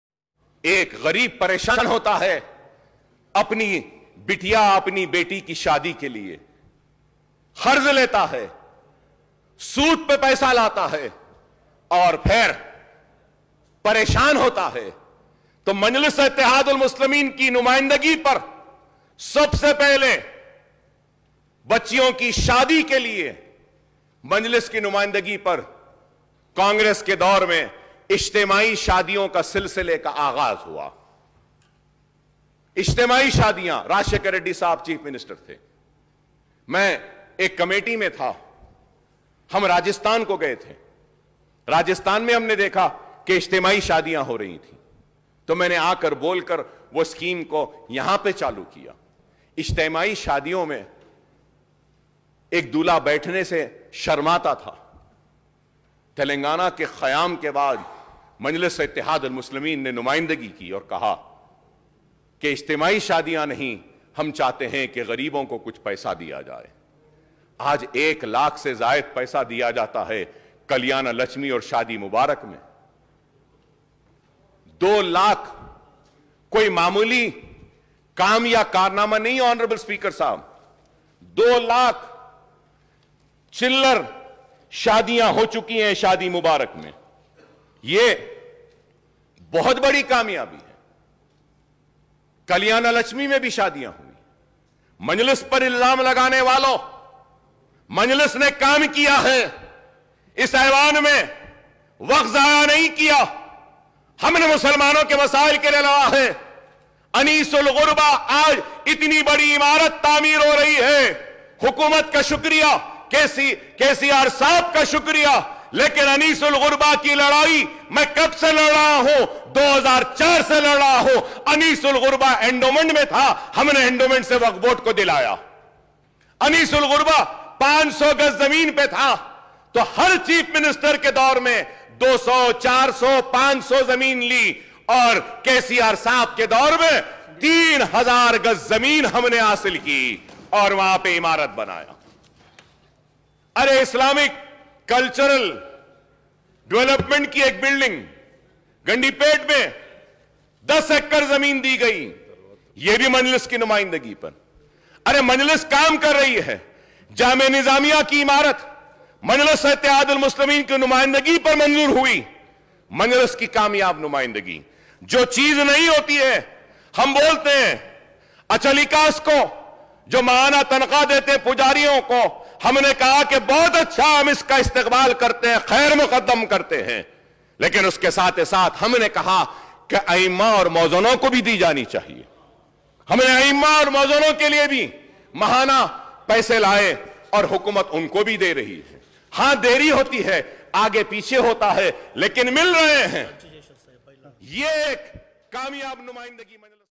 ओवैसी किया बहुत बड़ा काम तेलंगाना के मुख्यमंत्री KCR खूब ताली बजाया
ओवैसी किया बहुत बड़ा काम तेलंगाना के मुख्यमंत्री KCR खूब ताली बजाया @arariaaimimnews